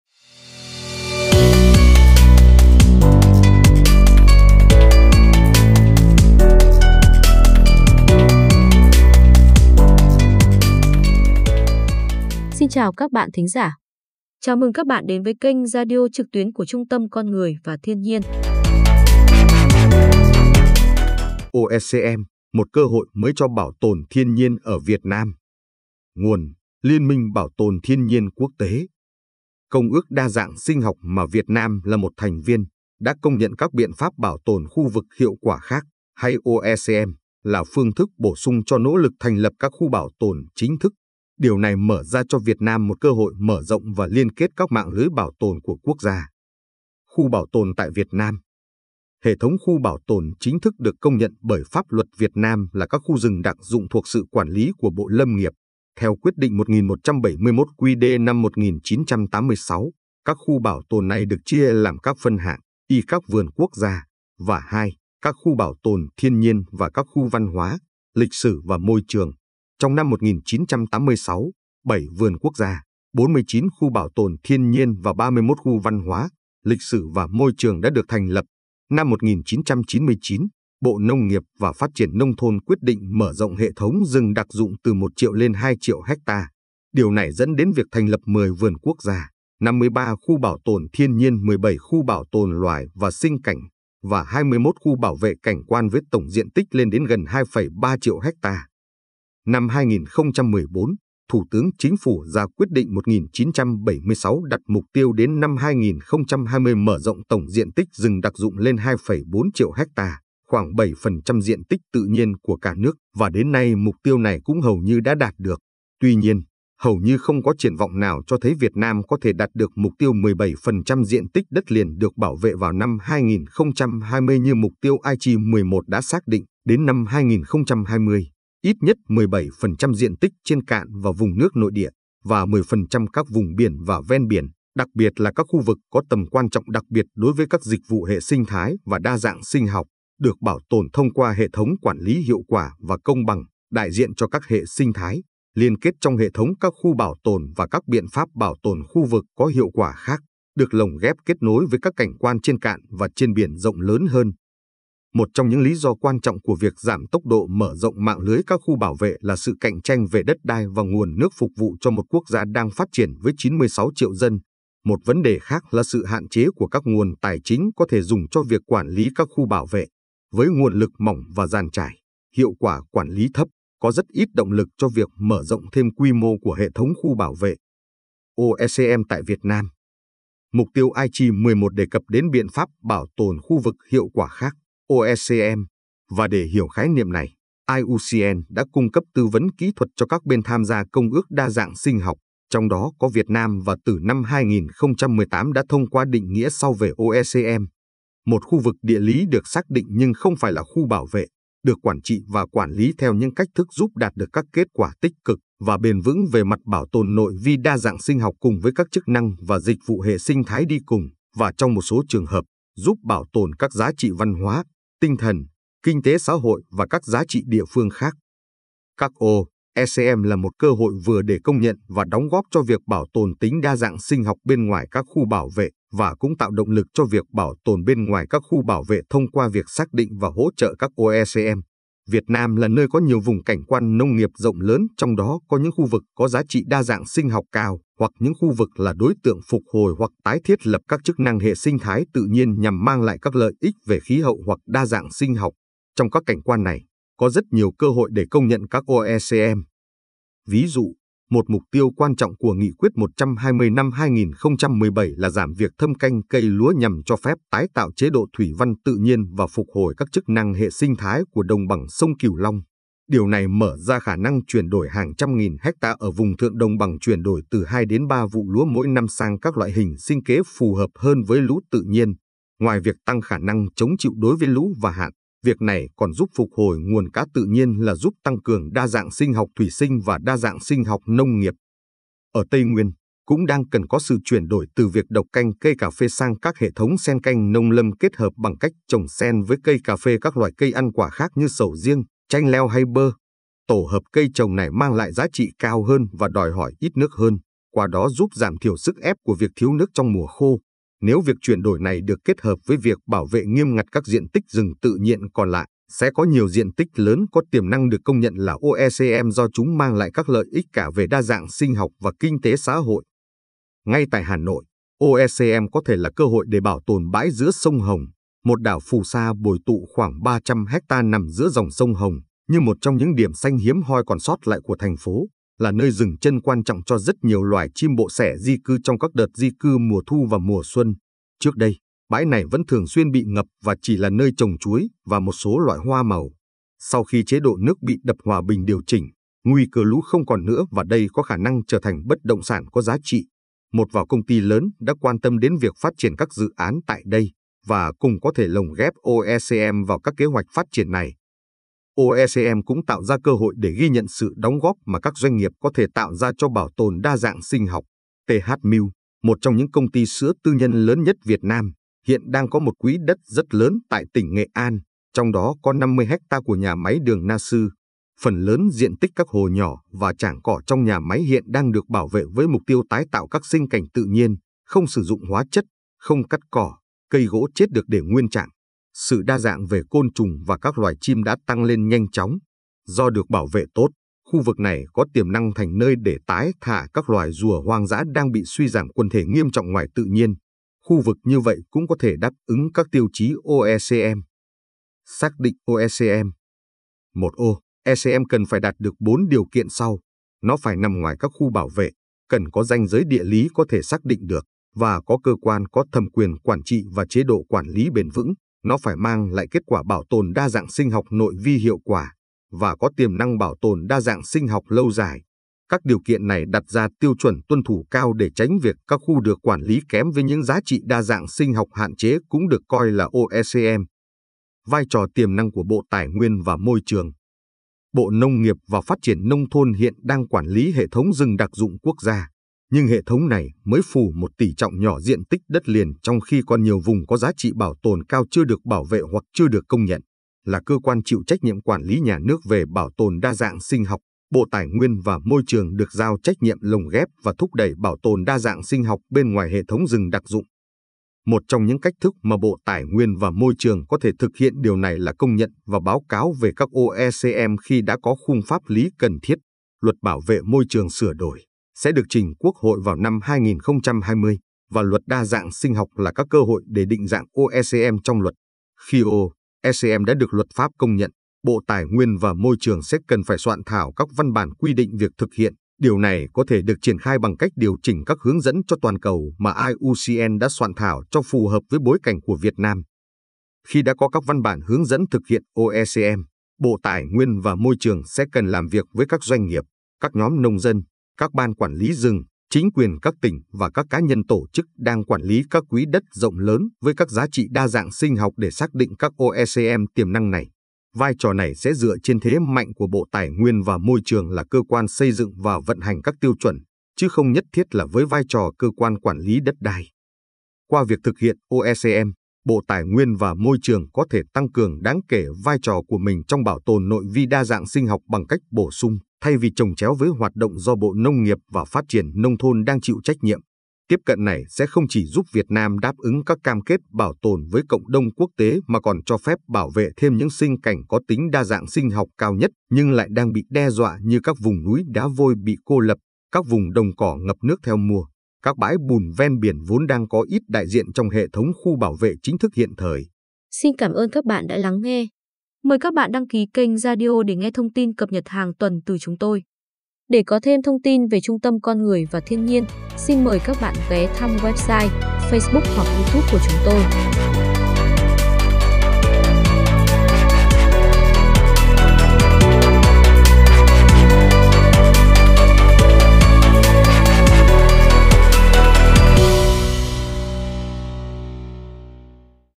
OECM: một cơ hội mới cho bảo tồn thiên nhiên ở Việt Nam
Công ước Đa dạng Sinh học mà Việt Nam là một thành viên, đã công nhận các “biện pháp bảo tồn khu vực hiệu quả khác” (hay OECM) là phương thức bổ sung cho nỗ lực thành lập các khu bảo tồn chính thức. Điều này mở ra cho Việt Nam một cơ hội mở rộng và liên kết các mạng lưới bảo tồn của quốc gia.